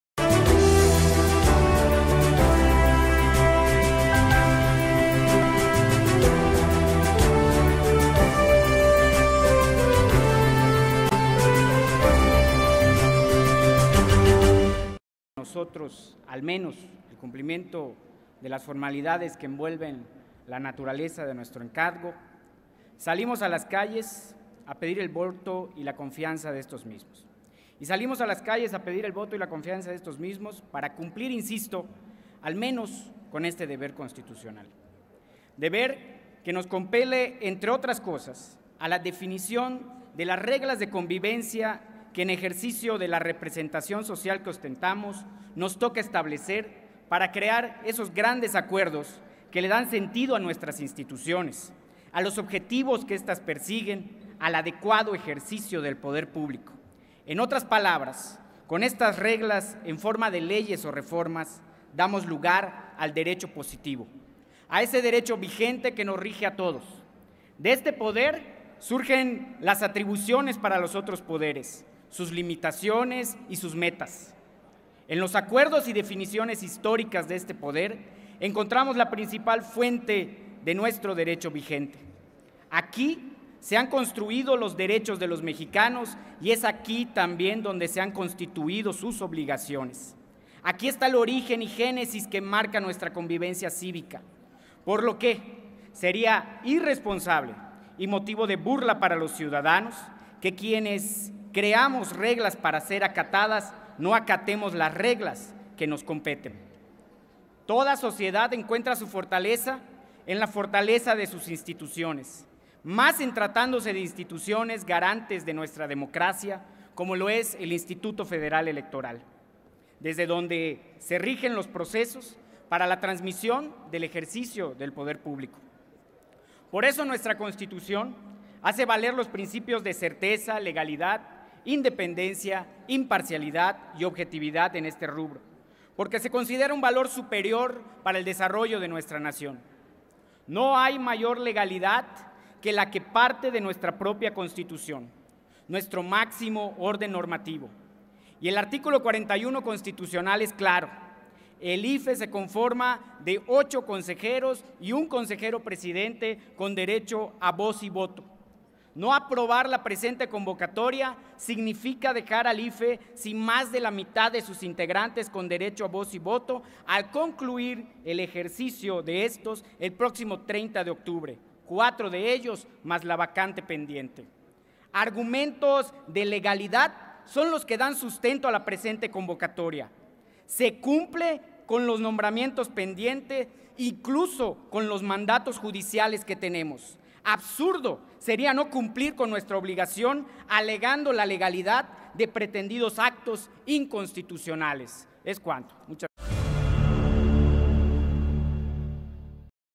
Dip. Mauricio Sahui (PRI) - Acuerdo JUCOPO (A Favor)
El Dip. Mauricio Sahui Rivero (PRI) señala que los representantes de la ciudadanía buscan el voto de los mexicanos para poner voz a sus reclamos y necesidades frente al Pleno de la Cámara de Diputados, necesidades que encontrarán respuestas mediante la concreción de acuerdos. Por lo cual resulta de suma trascendencia y relevancia aprobar el acuerdo con el fin de velar por la legitimidad de las instituciones, en este caso, del IFE.